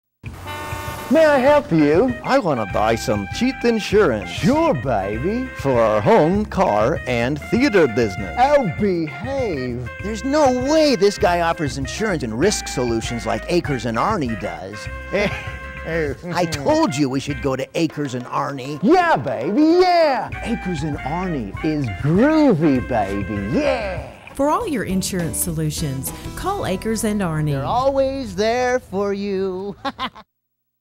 Jim Barber - Akers & Arney Insurance Spot #4
Akers & Arney Insurance Commercial
Character Host: Jim Barber
Directed by Jennifer McCullough
Video & Editing: The Vacation Channel - Branson, MO